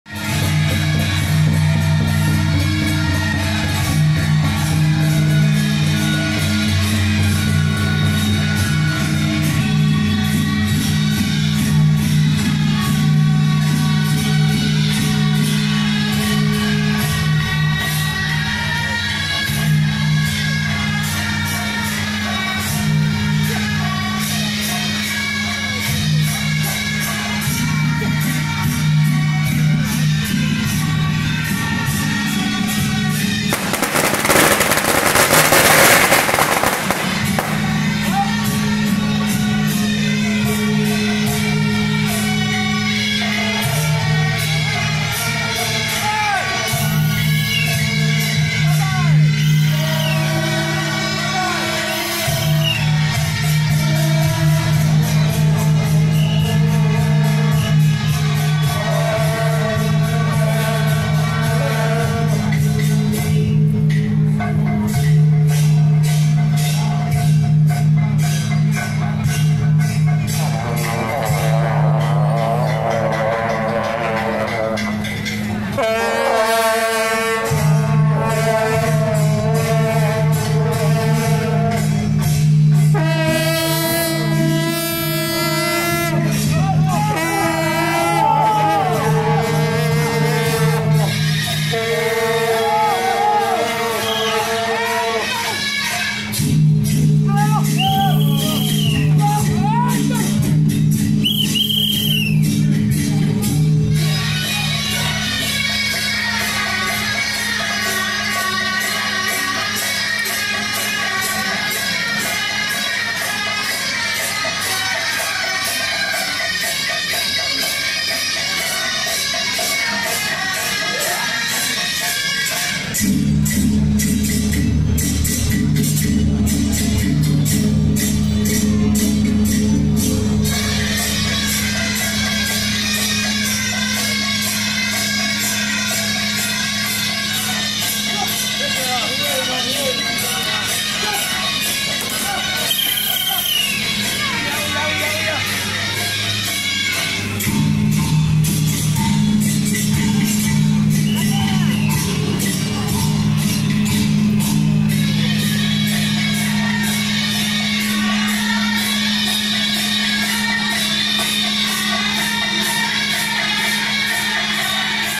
107新莊後港慈惠堂瑤池金母建堂60週年遶境【新竹天靈山慈惠母宮-瑤池金母 聖駕】
新竹天靈山慈惠母宮-瑤池金母 聖駕